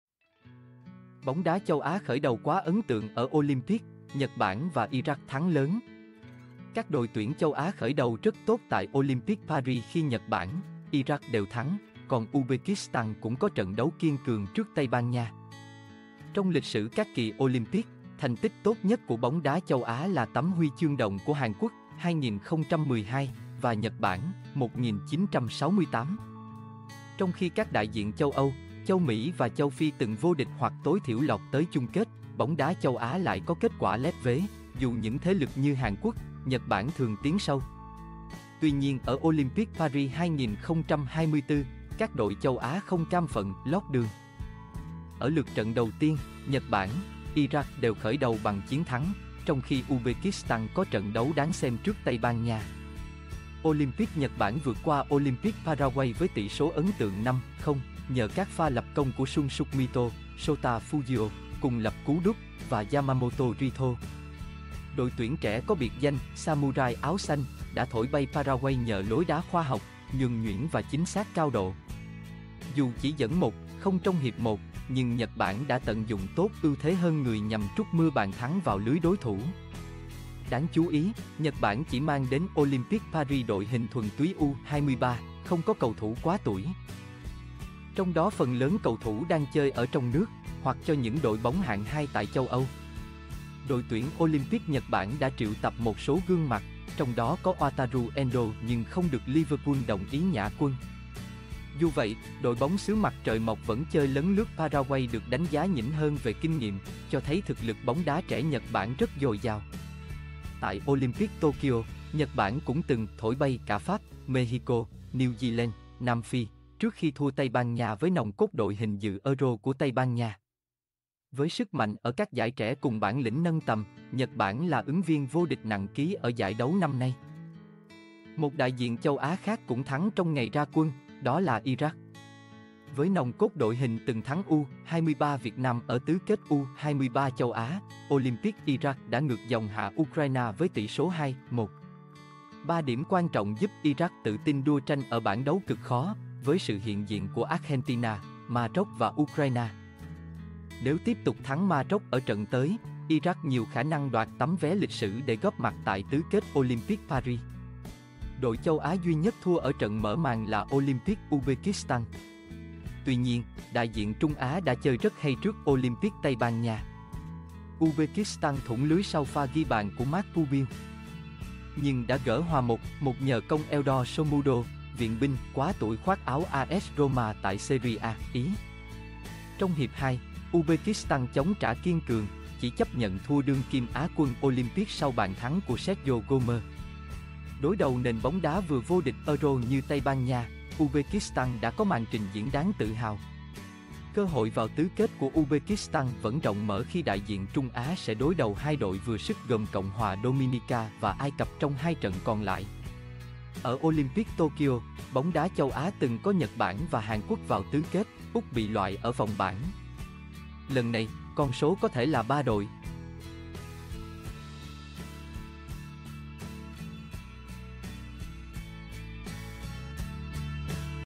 Bóng đá châu Á khởi đầu quá ấn tượng ở Olympic: Nhật Bản và Iraq thắng lớn
Các đội tuyển châu Á khởi đầu rất tốt tại Olympic Paris khi Nhật Bản, Iraq đều thắng, còn Uzbekistan cũng có trận đấu kiên cường trước Tây Ban Nha.
Trong lịch sử các kỳ Olympic, thành tích tốt nhất của bóng đá châu Á là tấm HCĐ của Hàn Quốc (2012) và Nhật Bản (1968). Trong khi các đại diện châu Âu, châu Mỹ và châu Phi từng vô địch hoặc tối thiểu lọt tới chung kết, bóng đá châu Á lại có kết quả lép vế, dù những thế lực như Hàn Quốc, Nhật Bản thường tiến sâu.

Tuy nhiên ở Olympic Paris 2024, các đội châu Á không cam phận "lót đường". Ở lượt trận đầu tiên, Nhật Bản, Iraq đều khởi đầu bằng chiến thắng, trong khi Uzbekistan có trận đấu đáng xem trước Tây Ban Nha.
Olympic Nhật Bản vượt qua Olympic Paraguay với tỷ số ấn tượng 5-0, nhờ các pha lập công của Shunsuke Mito, Shota Fujio (cùng lập cú đúp) và Yamamoto Rihito. Đội tuyển trẻ có biệt danh "Samurai áo xanh" đã thổi bay Paraguay nhờ lối đá khoa học, nhuần nhuyễn và chính xác cao độ. Dù chỉ dẫn 1-0 trong hiệp 1, nhưng Nhật Bản đã tận dụng tốt ưu thế hơn người nhằm trút mưa bàn thắng vào lưới đối thủ.
Đáng chú ý, Nhật Bản chỉ mang đến Olympic Paris đội hình thuần túy U.23 (không có cầu thủ quá tuổi), trong đó phần lớn cầu thủ đang chơi ở trong nước, hoặc cho những đội bóng hạng hai tại châu Âu. Đội tuyển Olympic Nhật Bản đã triệu tập một số gương mặt, trong đó có Wataru Endo nhưng không được Liverpool đồng ý nhả quân. Dù vậy, đội bóng xứ mặt trời mọc vẫn chơi lấn lướt Paraguay được đánh giá nhỉnh hơn về kinh nghiệm, cho thấy thực lực bóng đá trẻ Nhật Bản rất dồi dào.

Tại Olympic Tokyo, Nhật Bản cũng từng "thổi bay" cả Pháp, Mexico, New Zealand, Nam Phi, trước khi thua Tây Ban Nha với nòng cốt đội hình dự EURO của Tây Ban Nha. Với sức mạnh ở các giải trẻ cùng bản lĩnh nâng tầm, Nhật Bản là ứng viên vô địch nặng ký ở giải đấu năm nay.

Một đại diện châu Á khác cũng thắng trong ngày ra quân, đó là Iraq. Với nòng cốt đội hình từng thắng U.23 Việt Nam ở tứ kết U.23 châu Á, Olympic Iraq đã ngược dòng hạ Ukraine với tỷ số 2-1.
3 điểm quan trọng giúp Iraq tự tin đua tranh ở bảng đấu cực khó, với sự hiện diện của Argentina, Ma Rốc và Ukraine. Nếu tiếp tục thắng Ma Rốc ở trận tới, Iraq nhiều khả năng đoạt tấm vé lịch sử để góp mặt tại tứ kết Olympic Paris.

Đội châu Á duy nhất thua ở trận mở màn là Olympic Uzbekistan. Tuy nhiên, đại diện Trung Á đã chơi rất hay trước Olympic Tây Ban Nha. Uzbekistan thủng lưới sau pha ghi bàn của Marc Pubill, nhưng đã gỡ hòa 1-1 nhờ công Eldor Shomurodov - "viện binh" quá tuổi khoác áo AS Roma tại Serie A (Ý). Trong hiệp 2, Uzbekistan chống trả kiên cường, chỉ chấp nhận thua đương kim á quân Olympic sau bàn thắng của Sergio Gomez. Đối đầu nền bóng đá vừa vô địch EURO như Tây Ban Nha, Uzbekistan đã có màn trình diễn đáng tự hào.

Cơ hội vào tứ kết của Uzbekistan vẫn rộng mở khi đại diện Trung Á sẽ đối đầu 2 đội vừa sức gồm CH Dominica và Ai Cập trong 2 trận còn lại.

Ở Olympic Tokyo, bóng đá châu Á từng có Nhật Bản và Hàn Quốc vào tứ kết (Úc bị loại ở vòng bảng). Lần này, con số có thể là 3 đội?